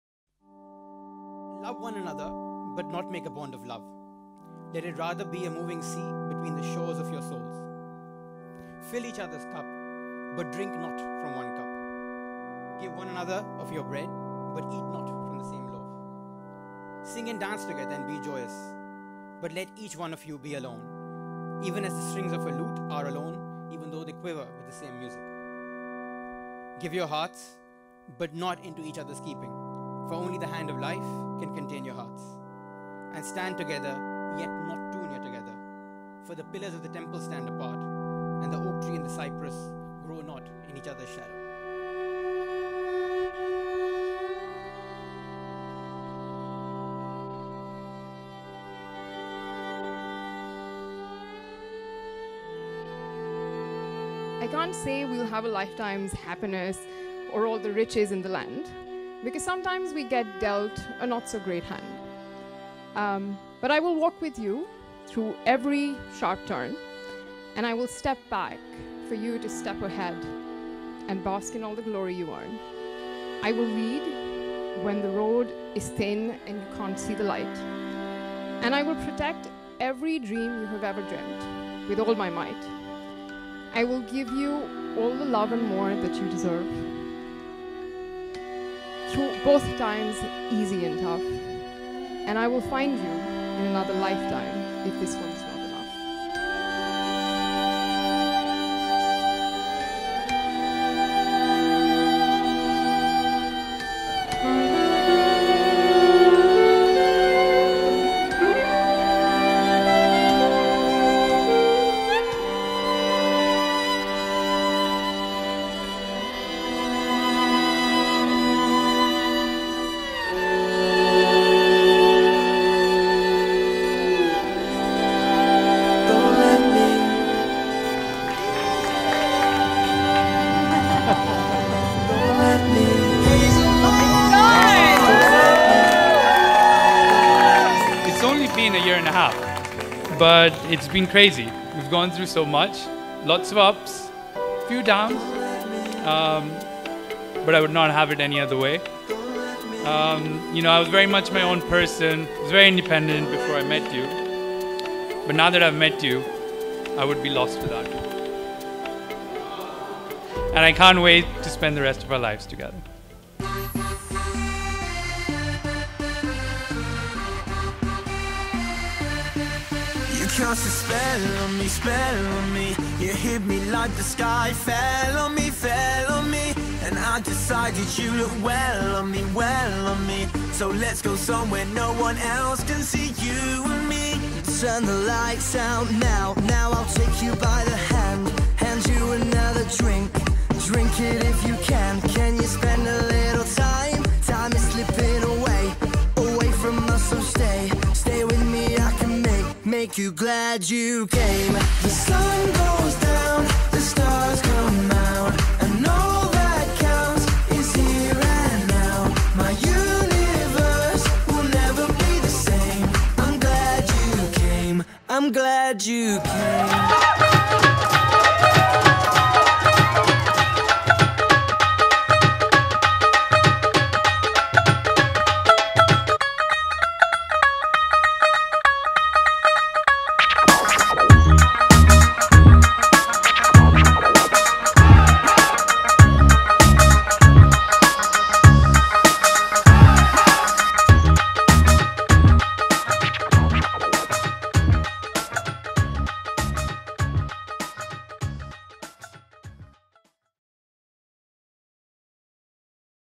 Indian Wedding at the Melograno Resort in Monopoli | Puglia, Italy
Shefali and Dhruwil are two very kind guys from London.
They choose beautiful Puglia to celebrate their wedding.
Melograno Resort has been a perfect frame for their celebration and has been a pleasure for us to be their videographer.
First Videographer: Matteo Di Maria
Aerial Shooting: Matteo Di Maria
Editing: MDM Wedding Videography
Photography: L&M Photography
Wedding Planner: Blooming Eventi

Camera Equipment: Sony Mirrorless
Drone: DJI Mavic Pro
Post production: Adobe Premiere CC